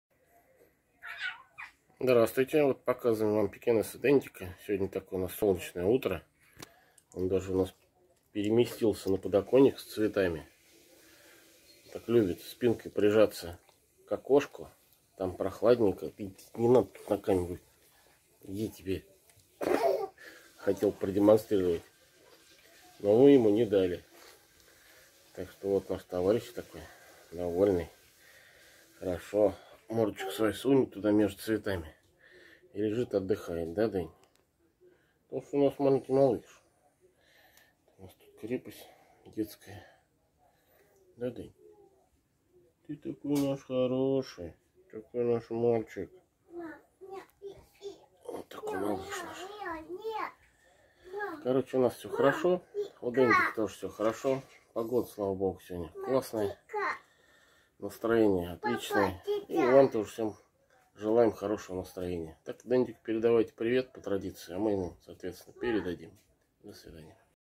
Пекинес Дендик любит лежать на подоконнике
Утро. Солнце в окошке. Пекинес Дендик тоже это ценит. И просится на подоконник, где полеживает и наслаждается прохладой от окна)))